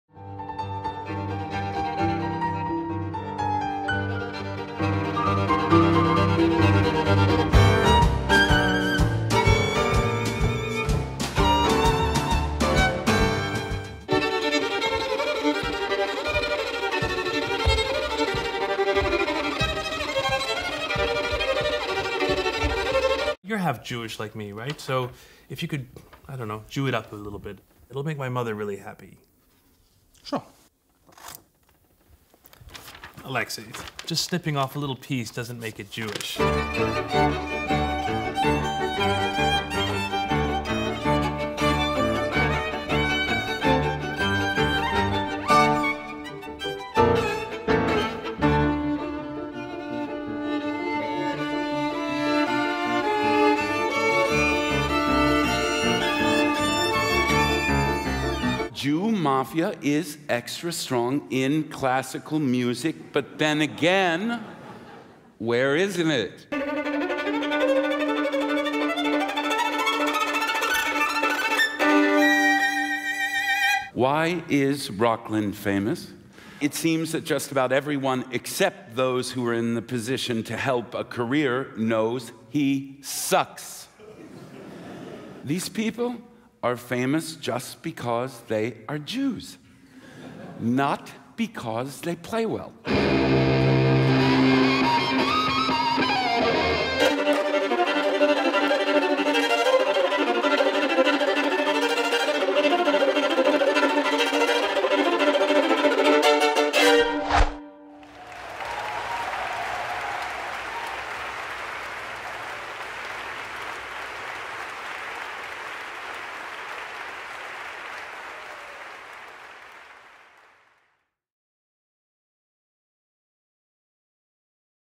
"Nicht schon wieder ein jüdischer Geiger!" - Igudesman beim Jüdischen Filmfestival Wien 2016
Trailer für Aleksey Igudesmans Konzert im Rahmen des Jüdischen Filmfestivals am 28. November 2016 um 19:00 im Gartenbaukino.